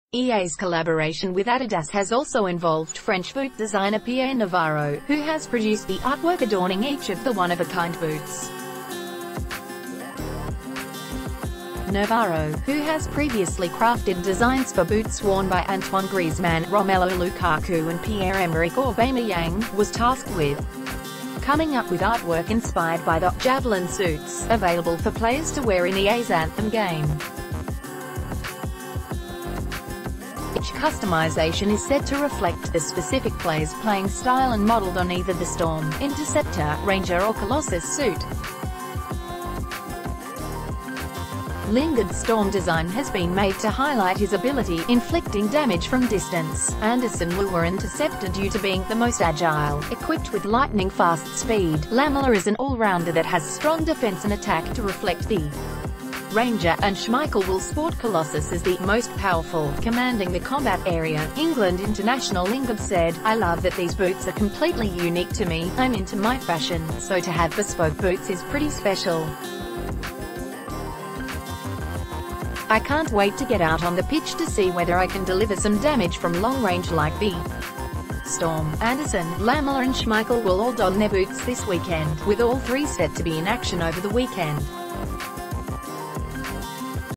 EA Sports release video-game themed boots
EA's collaboration with Adidas has also involved French boot designer Pierre Navarro, who has produced the artwork adorning each of the one-of-a-kind boots. Navarro, who has previously crafted designs for boots worn by Antoine Griezmann, Romelu Lukaku and Pierre-Emerick Aubameyang, was tasked with c...